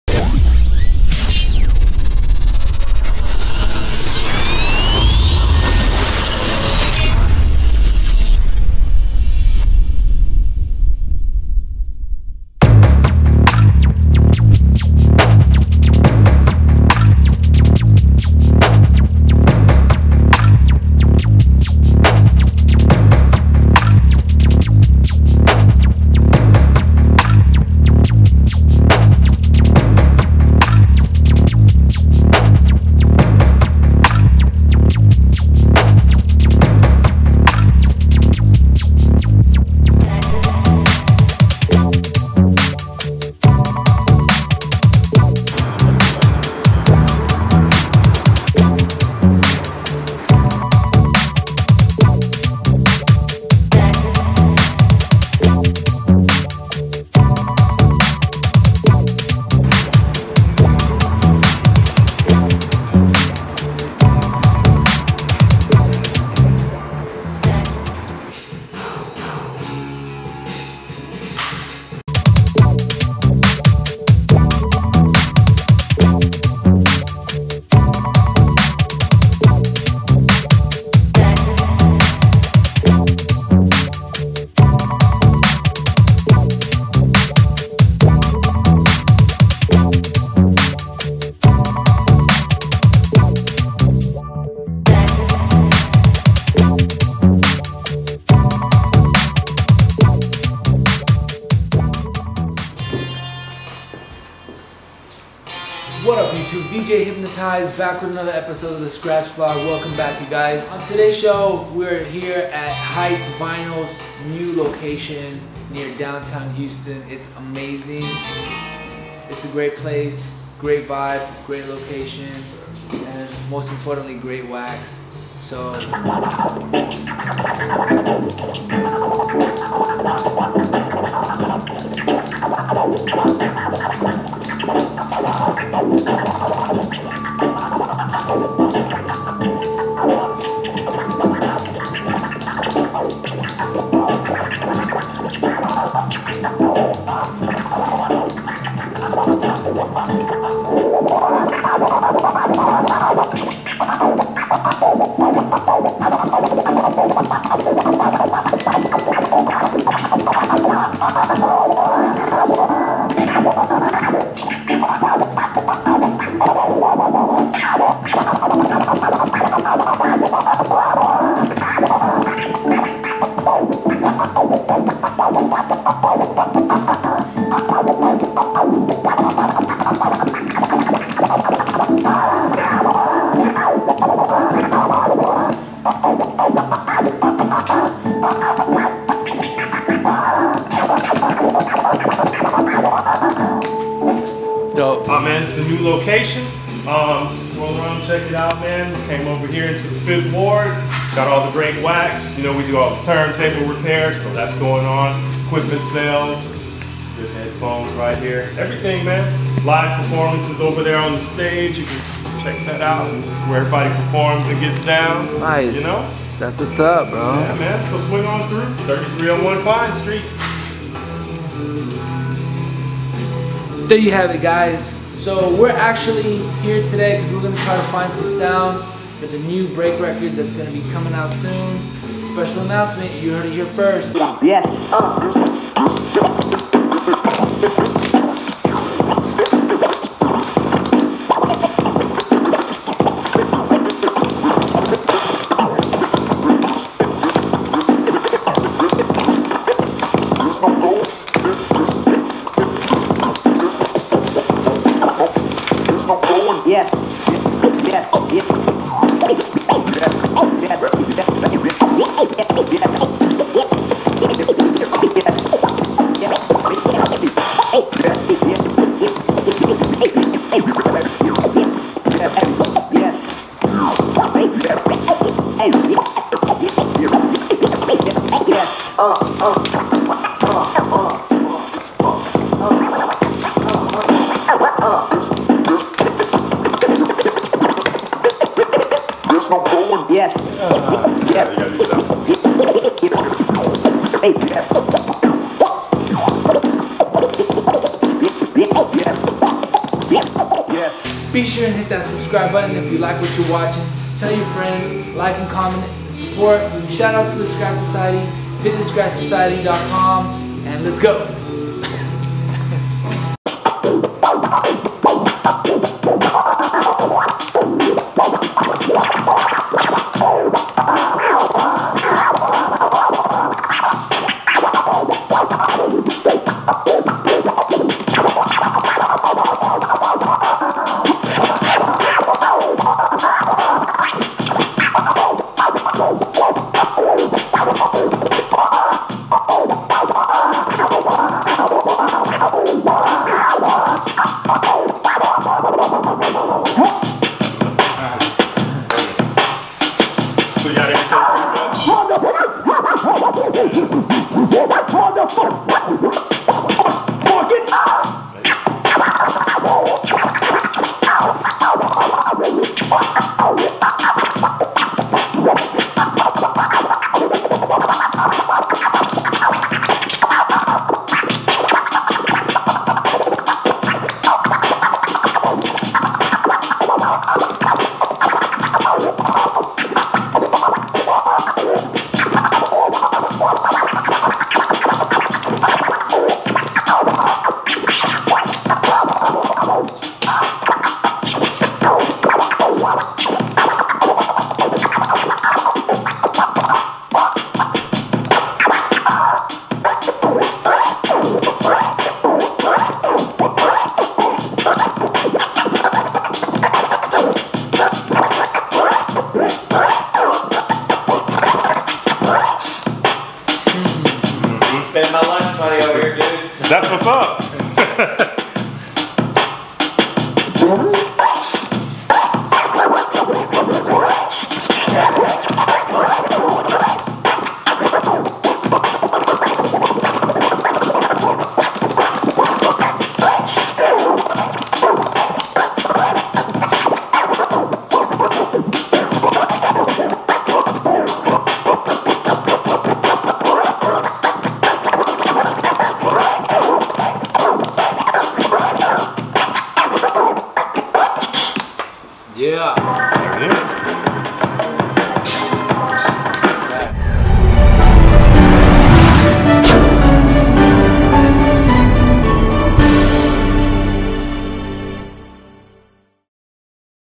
Skratch VLog Ep 81 "Easter Cuts" - Skratch Society Heights Vinyl Portablist Shredder Edition
Skratch Society Skratch Vlog ep 81 - Shredding at Heights vinyl new location near downtown Houston feat TKNC .  Watch for a major announcement as we are preparing for our next big project and leave a like and comment and subscribe for more scratching, portablism and turntablist premier content, and more!  Thanks for the continued support !!  ~H